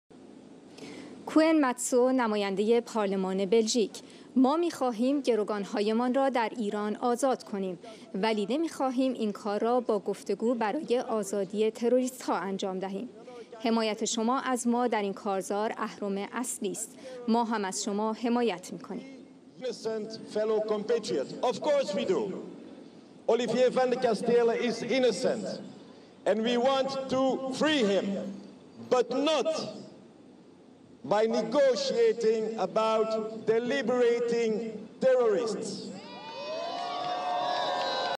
کوئن متسو نماینده پارلمان بلژیک: حمایت شما از ما در این کارزار اهرم اصلی است ـ ۲۸تیر۱۴۰۱
کوئن متسو نماینده پارلمان بلژیک: ما می‌خواهیم گروگانهایمان را در ایران آزاد کنیم  ولی نمی‌خواهیم اینکار را با گفتگو برای آزادی تروریستها انجام دهیم. حمایت شما از ما در این کارزار اهرم اصلی است. ما هم از شما حمایت می‌کنیم.

سیمای آزادی ۲۸ تیر۱۴۰۱